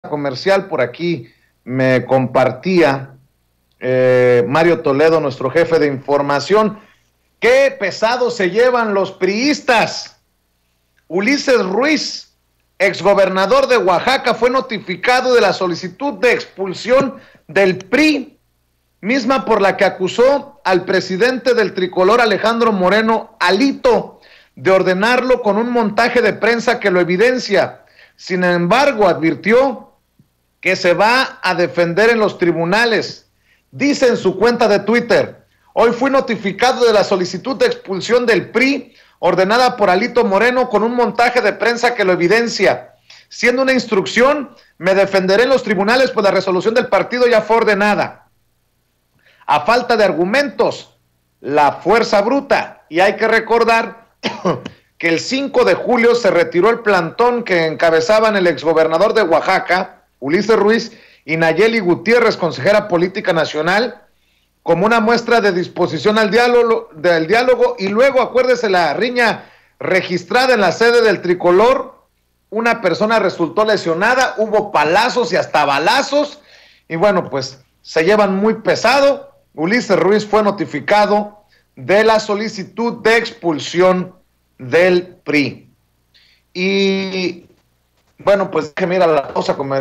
ULISES RUIZ DICE QUE ALITO LO EXPULSA DEL PRI… PERO QUE SE VA A DEFENDER EN TRIBUNALES
El exgobernador de Oaxaca aseguró que fue notificado de su expulsión del PRI ordenada por Alito Moreno, pero que se va a defender en los tribunales